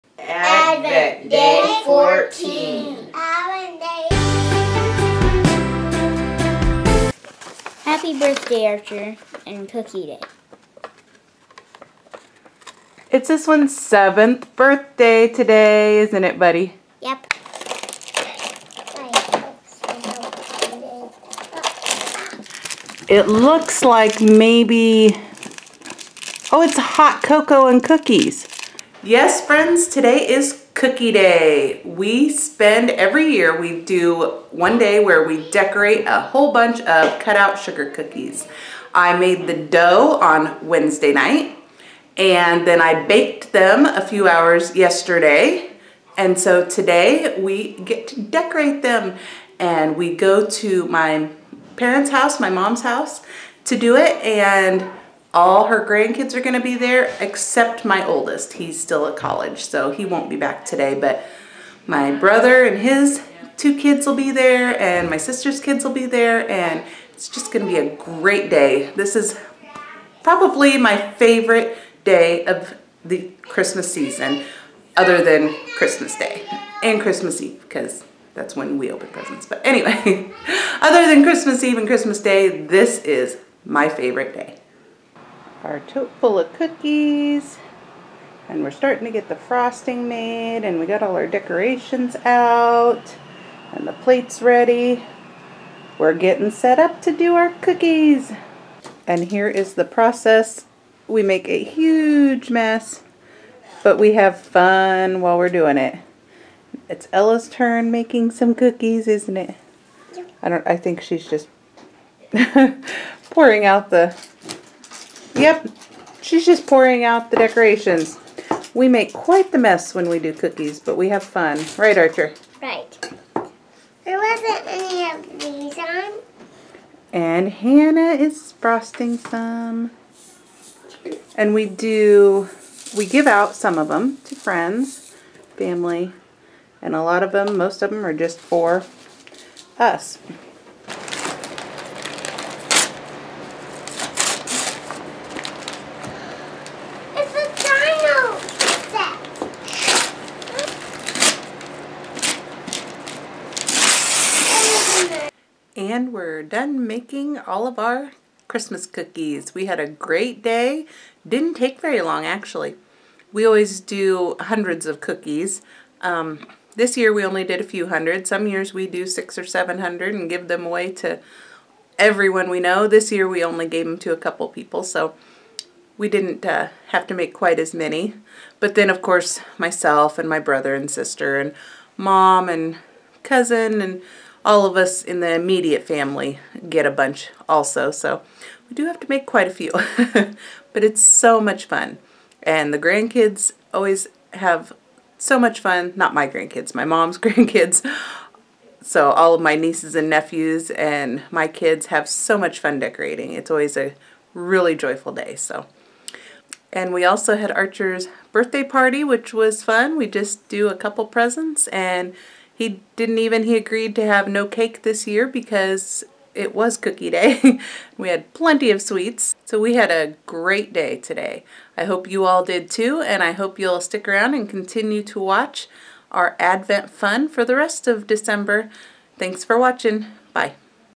Advent Dec. 14- Christmas Cookie Decorating Day!
A favorite we do every year. Family, fun, and cookies, what could be better.
Merry Christmas!